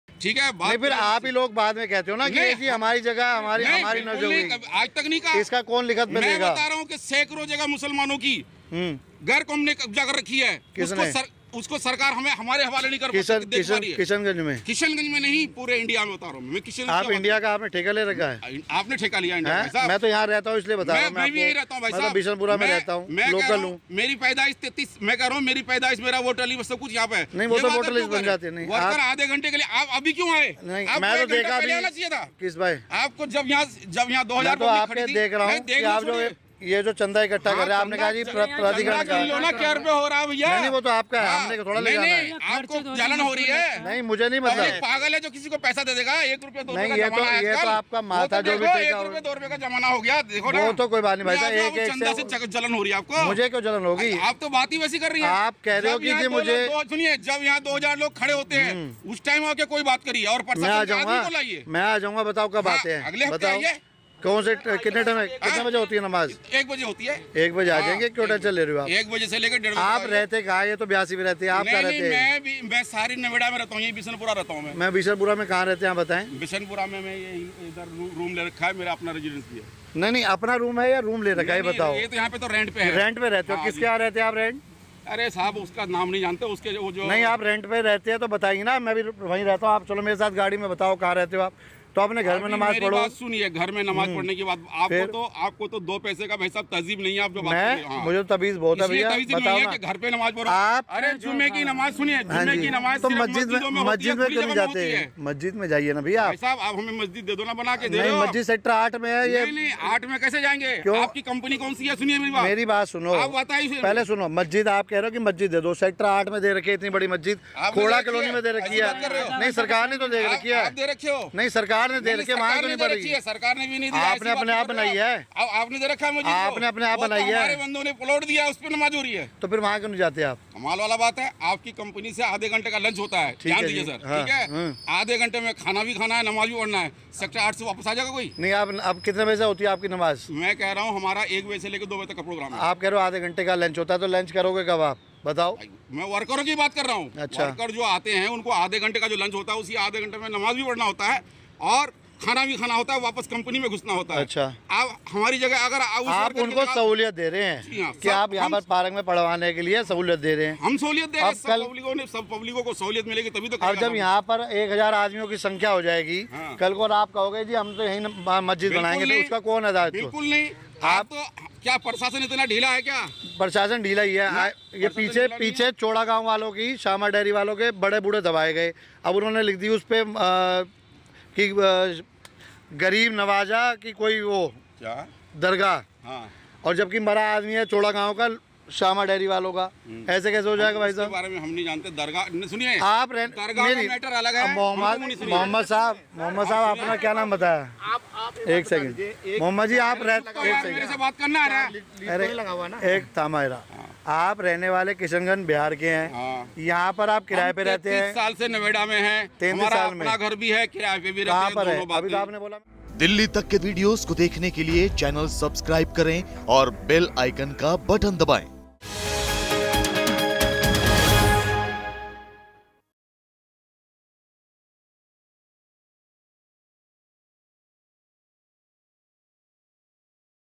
नमाज़ नहीं पढ़ने देने पर इमाम और स्थानीय में हुई बहसबाज़ी का देखें viral video...| Dilli Tak
नोएडा के पार्क में नमाज पर पुलिस की रोक के बावजदू बवाल थमता नहीं दिख रहा है. अब असदुद्दीन ओवैसी मैदान में कूद पड़े हैं. ओवैसी ने ट्वीट करके कहा कि एक तरफ तो पुलिस वाले हेलीकॉप्टर से कांवडियों पर फूल बरसाते हैं और दूसरी तरफ हफ्ते में एक वक्त की नमाज से कानून-व्यवस्था और शांति को खतरा पैदा हो जाता है. इस बीच एक नया वीडियो भी सामने आया है, जिसमें नमाज से पहले इमाम और वीडियो बना रहे लोगों को बीच तनातनी हो जाती है.